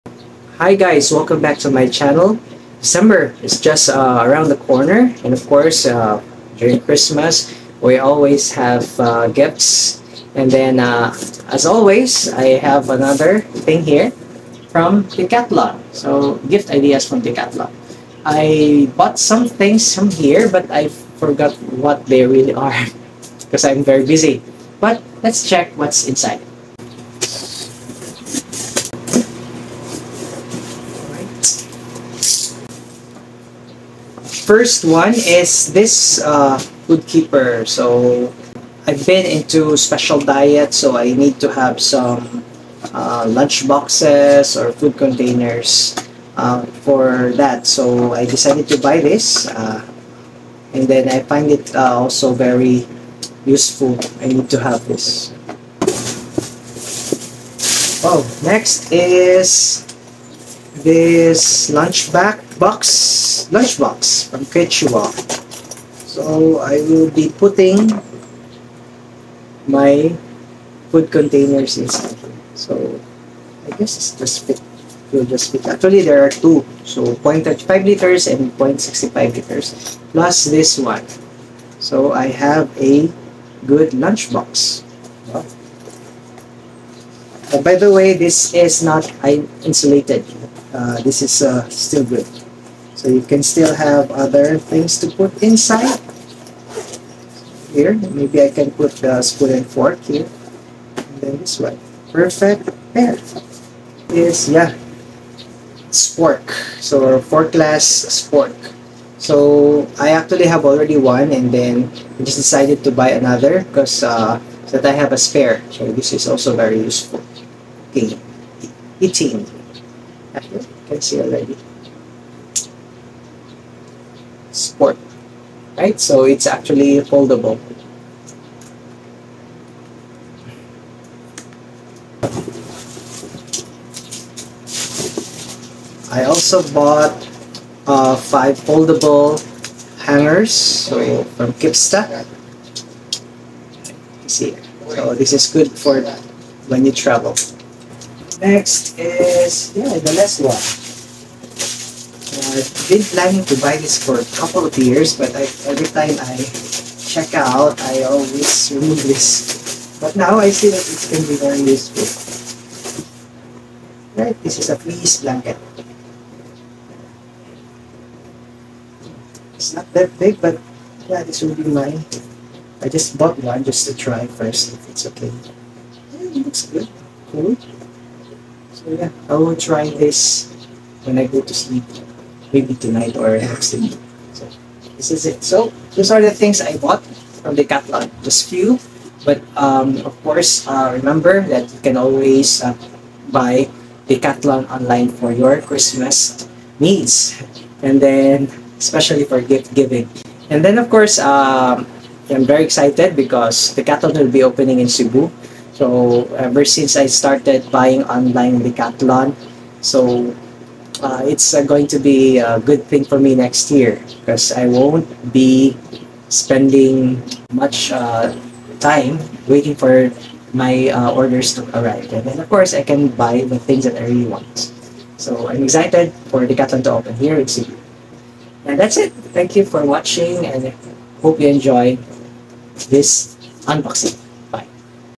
Christmas Gift Ideas from Decathlon 2023
It's Christmas season. Check out some great finds for your gift ideas only at Decathlon.